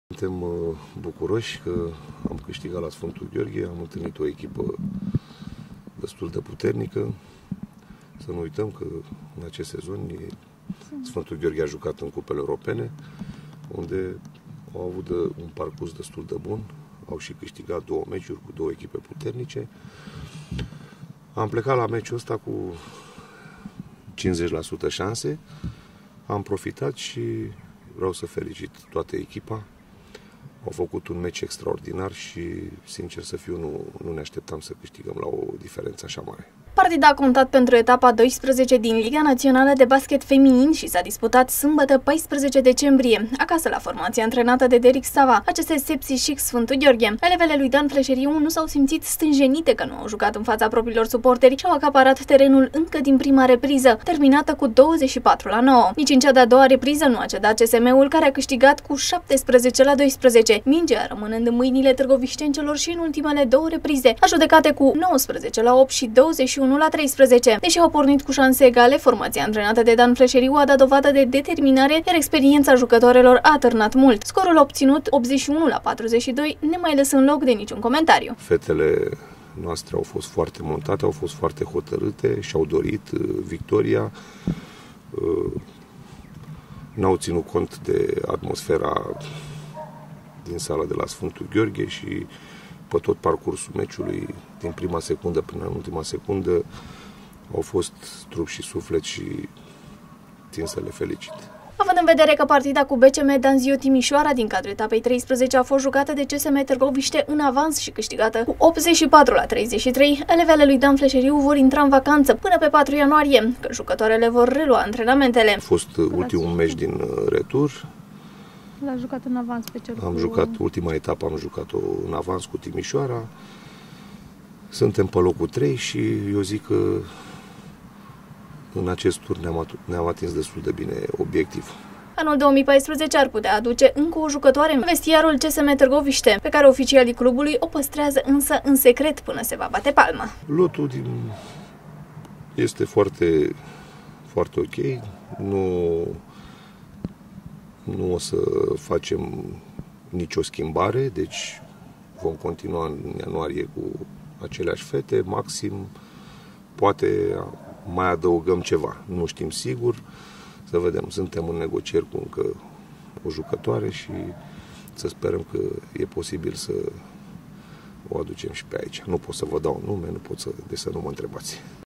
CSM Târgoviște la două puncte distanță de fotoliul de lider (Columna TV)
Echipa de baschet CSM Târgoviște este din nou pe val. Așa cum îi stă bine unei foste campioane fiecare meci bifat conturează câte o victorie răsunătoare. Ultima victimă a elevelor lui Dan Fleșeriu a fost ACS Sepsi SIC Sf. Gheorghe, singura reprezentată în actualul sezon al EuroCup Women care și-a cedat propiul teren în fața scorului de 81-42.